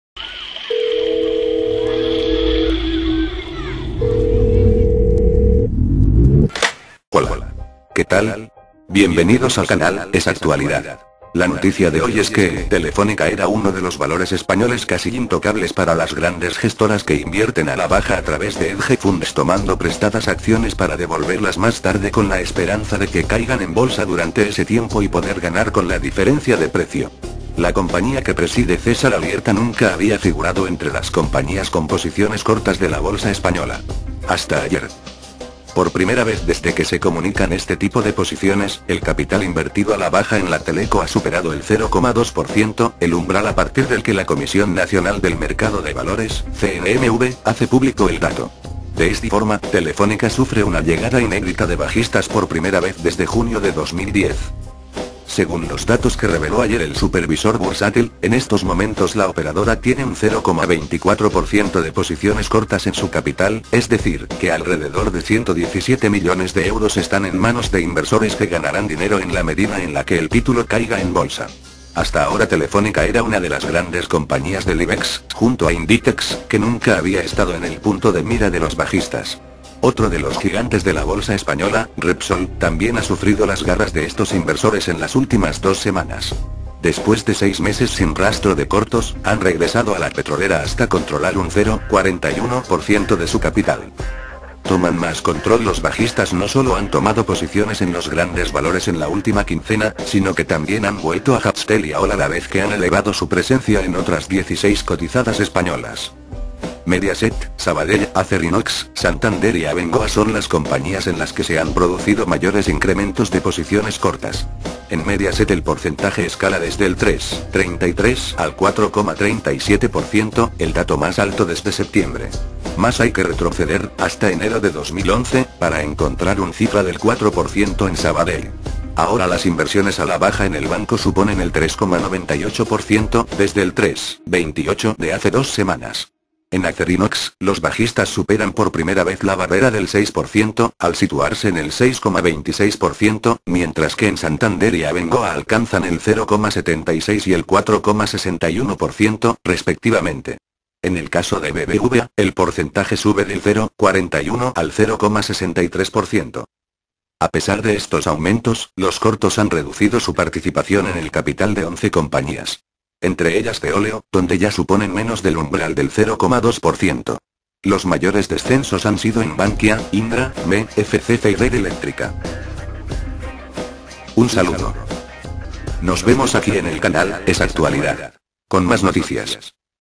Telefónica sufre una llegada inédita de posiciones bajistas
Texto de la noticia en ESActualidad :Telefónica era uno de los valores españoles casi intocables para las grandes gestoras que invierten a la baja a través de hedge funds tomando prestadas acciones para devolverlas más tarde con la esperanza de que caigan en bolsa durante ese tiempo y poder ganar con la diferencia de precio. La compañía que preside César Alierta nunca había figurado entre las compañías con posiciones cortas de la bolsa española. Hasta ayer